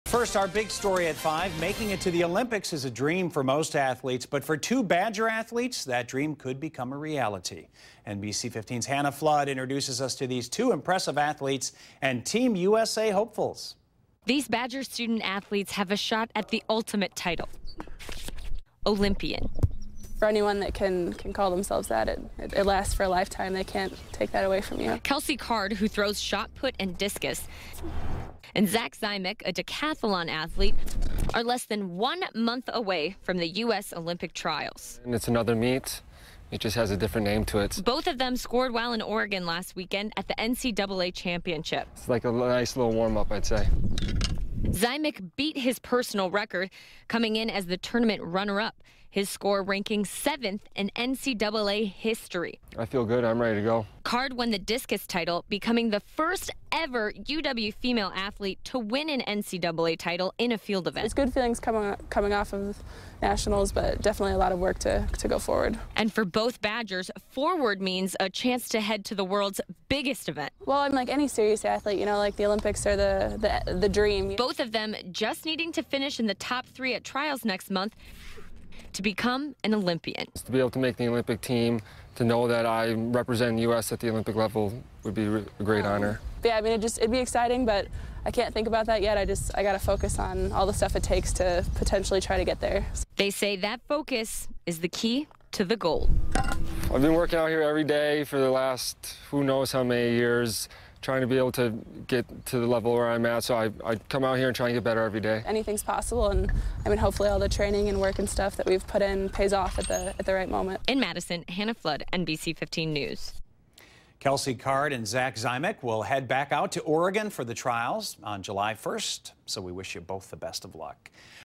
UW-Madison athletes head to Olympic trials
Making it to the Olympics is a dream for most athletes, but for two Badger athletes that dream could become a reality.